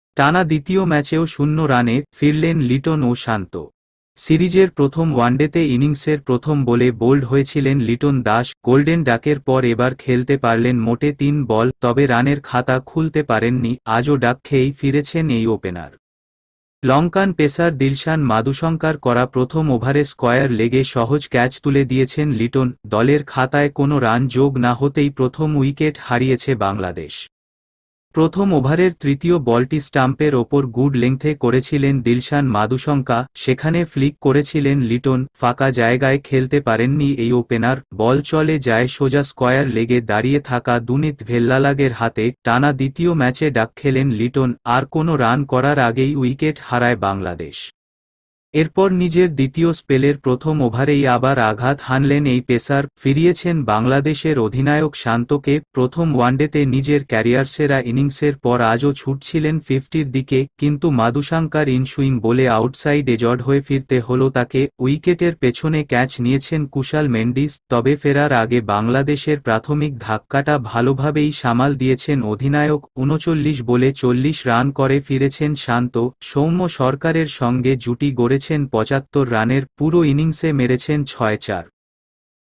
টানা দ্বিতীয় ম্যাচে শূন্য করে ফিরল লিটন
টানা দ্বিতীয় ম্যাচে শূন্য লিটনের, ফিরলেন শান্তও

 সিরিজের প্রথম ওয়ানডেতে ইনিংসের প্রথম বলে বোল্ড হয়েছিলেন লিটন দাস। গোল্ডেন ডাকের পর এবার খেলতে পারলেন মোটে ৩ বল।